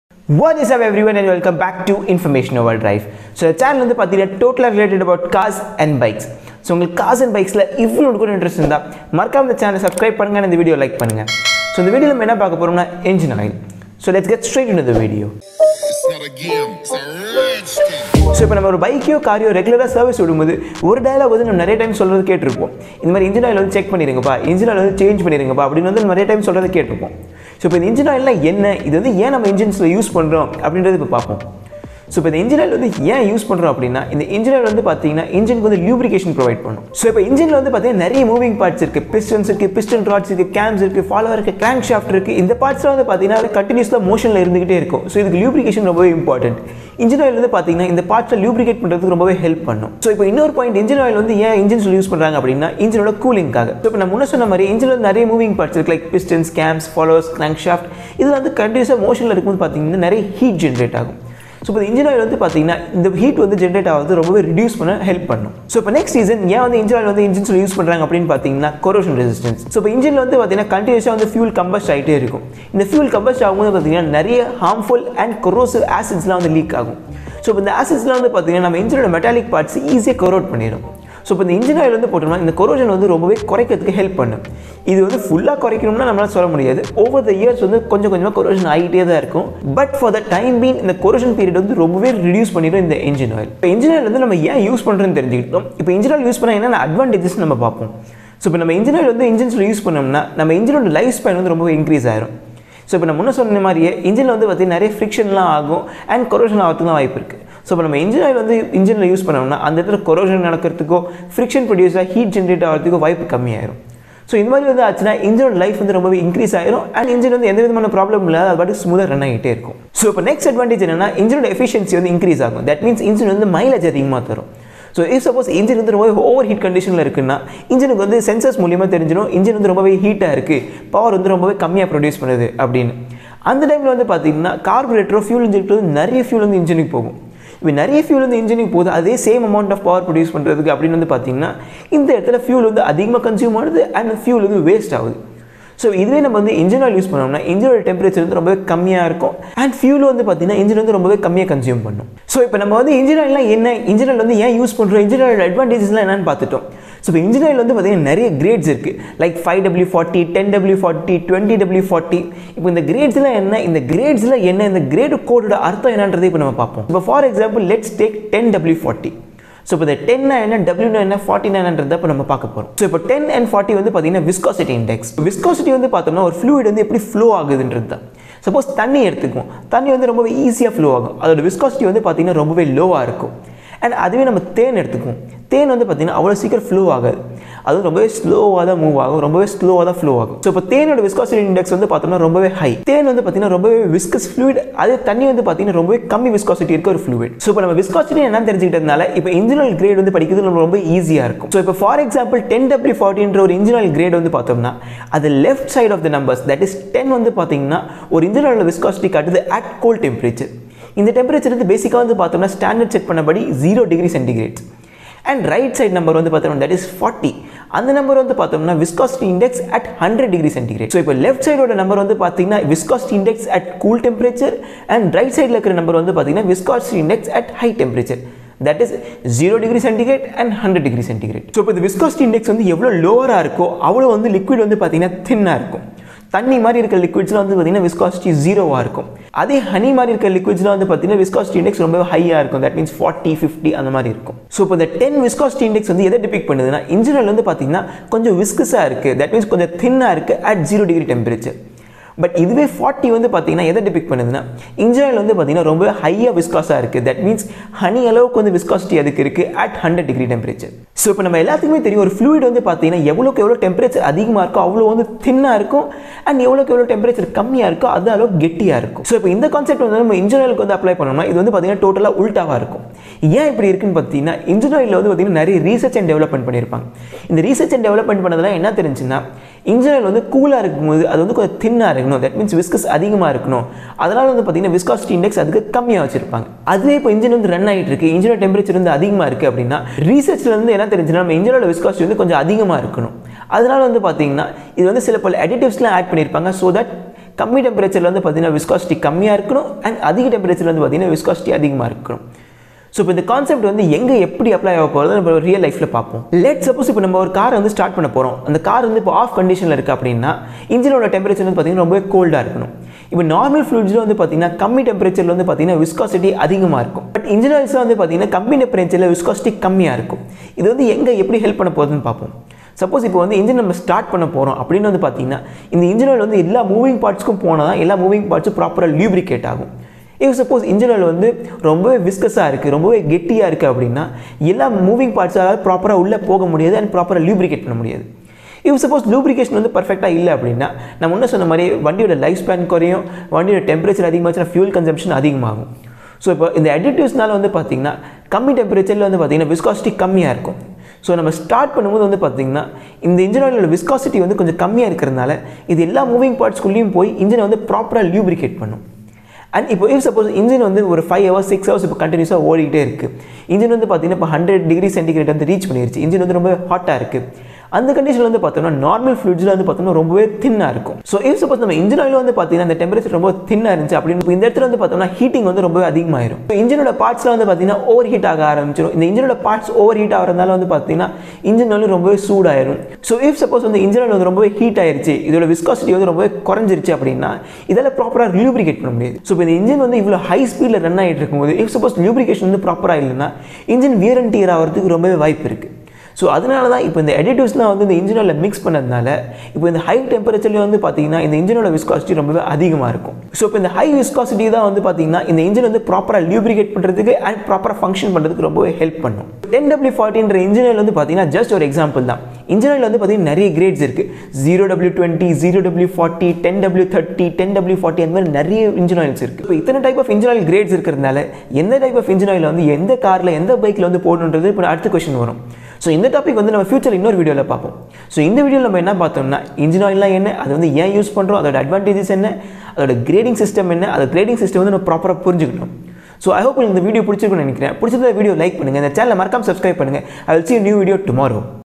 Engine Oil Grades Explained !!! | Why Engine Oil is used ?
HI everyone this is Gokul this side  in this video I have shared about engine oils ,why engine oil is used its advantages and i have explained  the grading system of engine oil